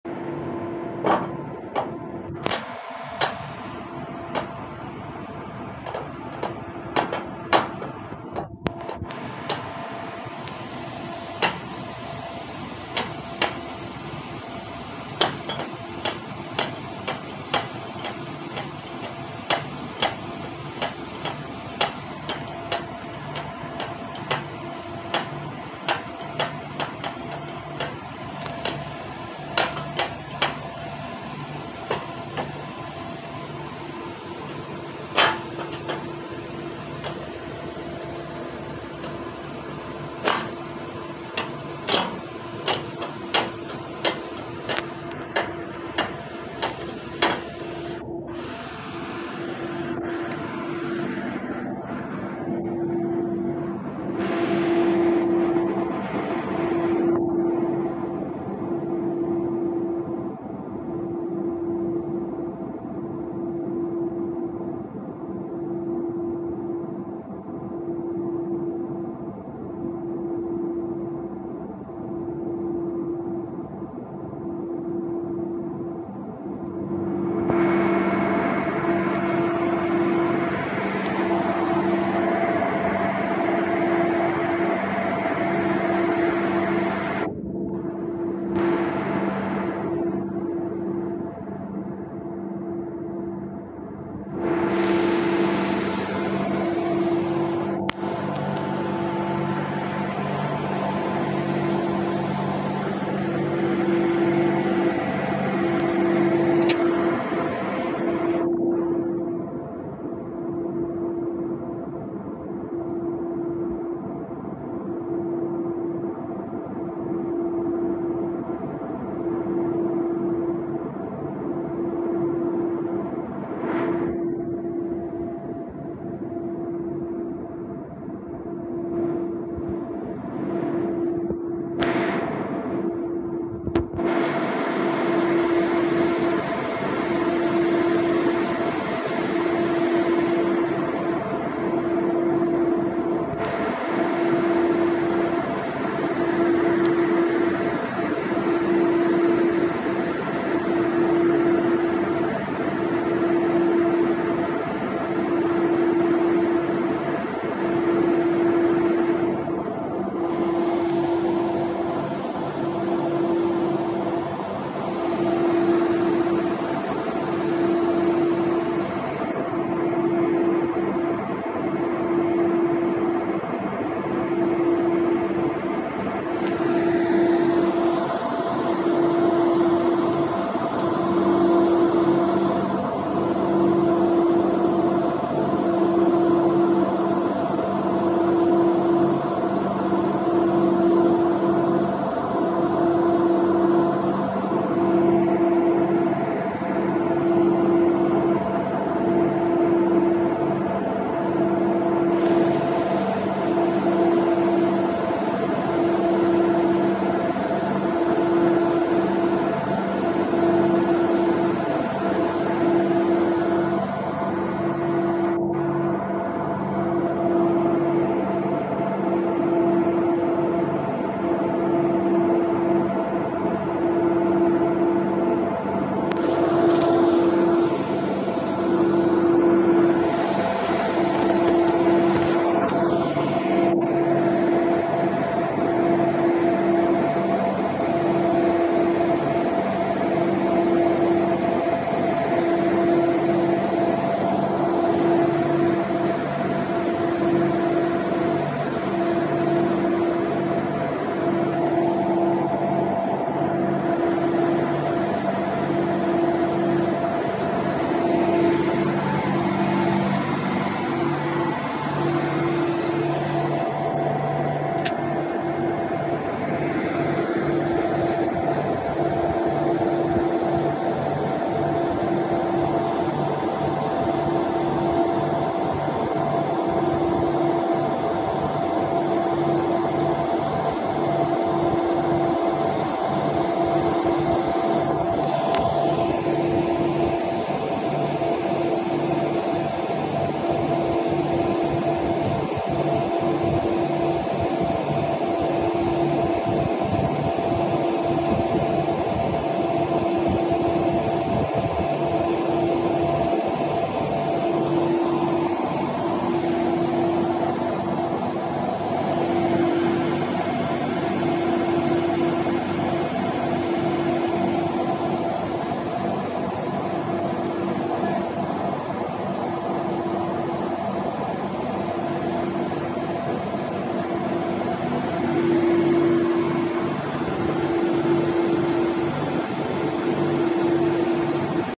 صيد ضخم في أعالي البحار: سحب شباك مليئة بالأسماك على متن سفينة صيد
الوصف: شاهد هذا الفيديو المذهل حيث تقوم سفينة صيد عملاقة بعملية صيد ضخمة في أعالي البحار والمحيطات! تتضمن اللقطات الحصرية سحب شباك الصيد المحملة بكميات هائلة من الأسماك، مما يظهر القوة والجمال الحقيقي لعالم الصيد البحري. استمتع بمشاهدة مراحل الصيد المختلفة والتحديات التي يواجهها الصيادون أثناء العمل في أعماق المحيطات.

بالطبع! إليك بعض الهاشتاجات التي يمكن أن تساعد في جذب المشاهدات على يوتيوب: